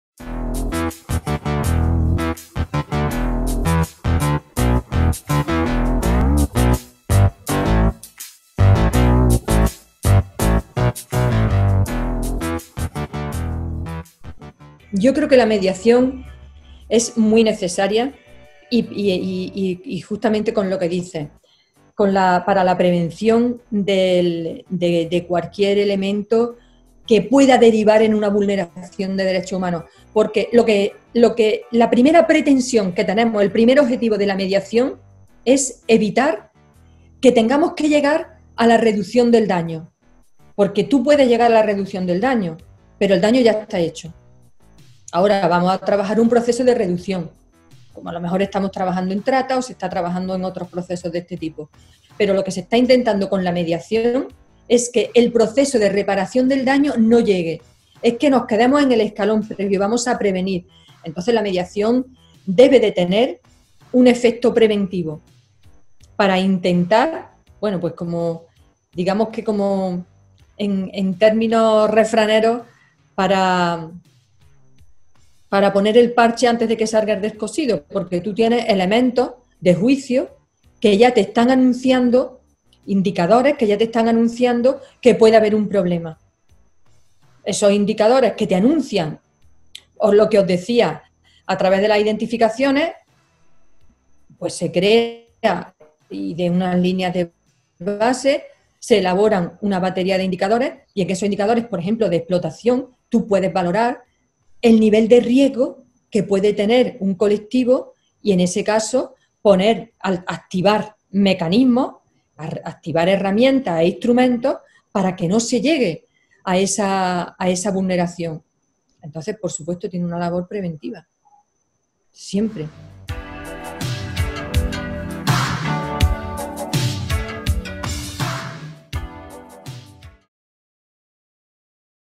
Píldoras Informativas: El trabajo en Huelva (6)
Sexto vídeo acerca de los antecedentes, la trayectoria y el trabajo de MZC en los campos de Huelva.